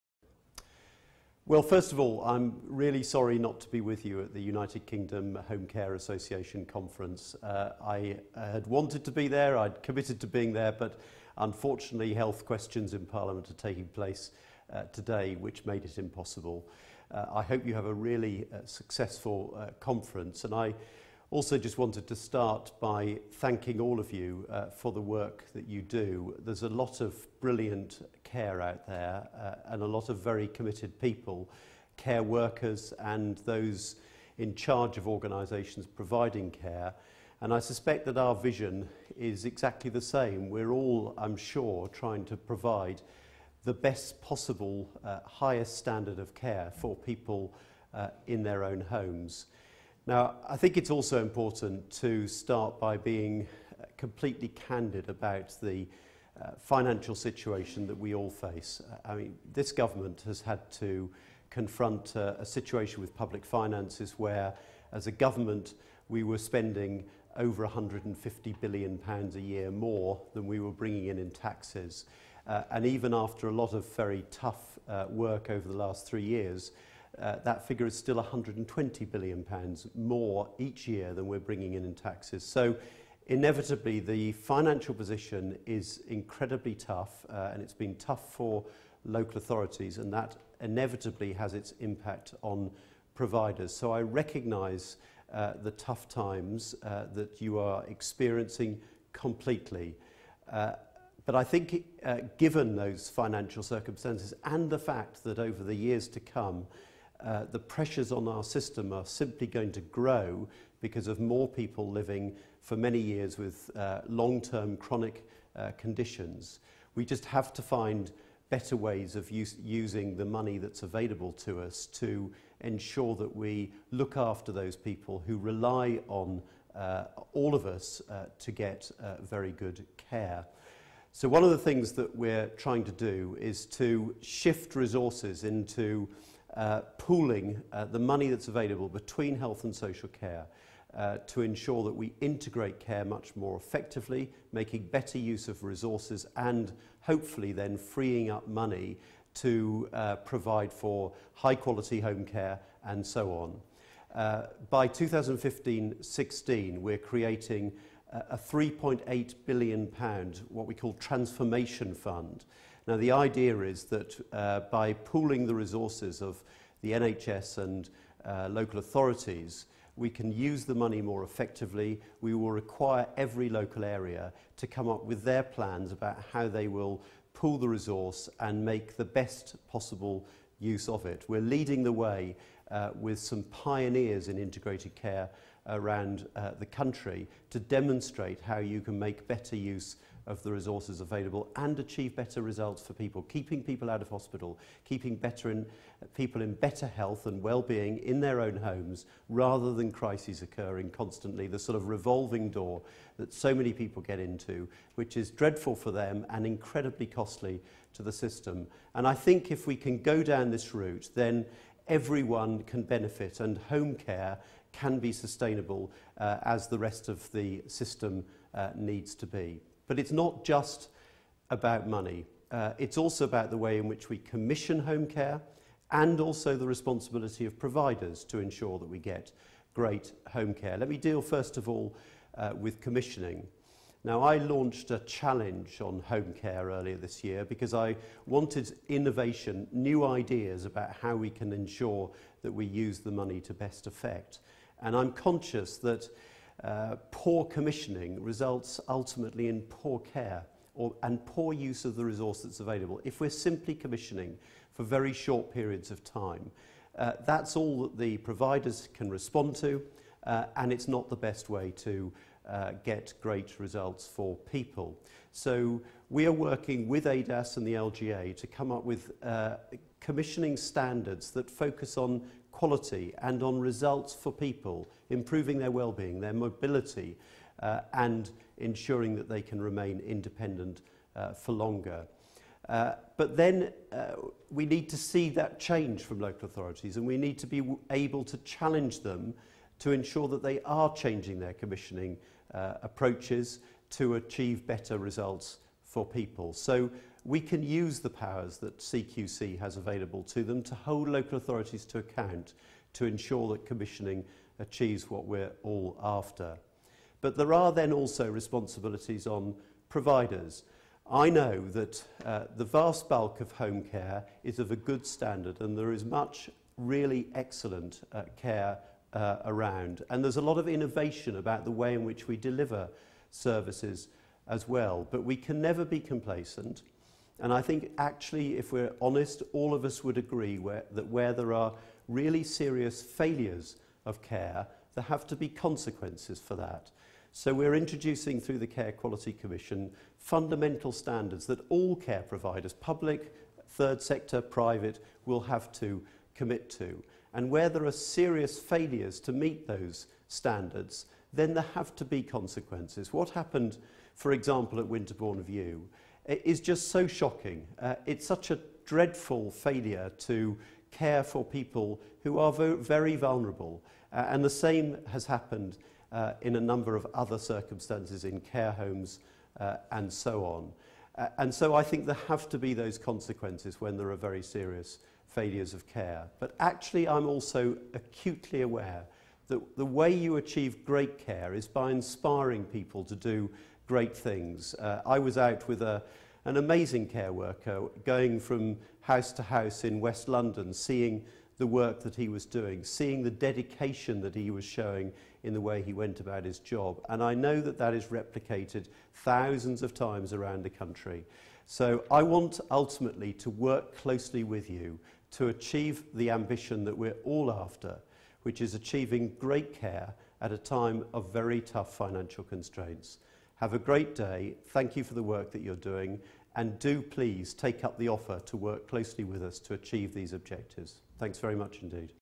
Norman Lamb's speech at the UKHCA England Conference 2013
Norman Lamb, Minister of State for Care & Support, speech at the UKHCA England Conference 2013